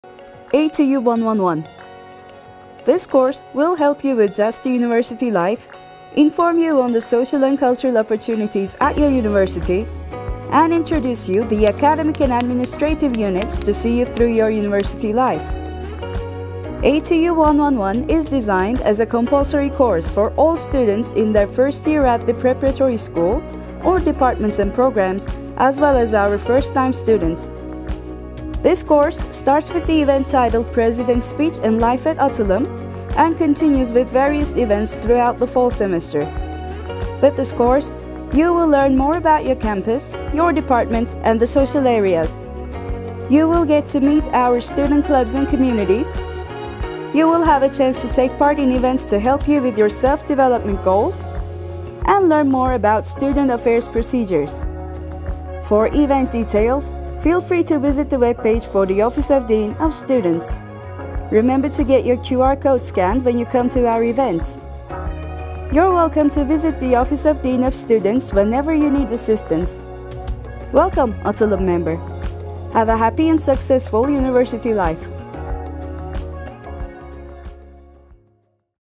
ATU111 Orientation
Atılım University
ATU100/ ATU111 are courses that are designed with the aim to provide our associate and undergraduate degree program students with more information about university life in general, and about Atılım University in particular. These courses constitute general orientation sessions for our new students. Students of the courses ATU 100/ ATU 111 learn more about university life, as well as the opportunities and the services accessible to them at Atılım University.

The courses ATU 100/ ATU 111 each have the credit value of 1, and are for all students. All new students of the university (including our Preparatory Class students) are required to take one of these courses. Preparatory Class students having taken one of these courses are to have the course coded for their first semester at their Department, at the end of which their existing grades will be logged.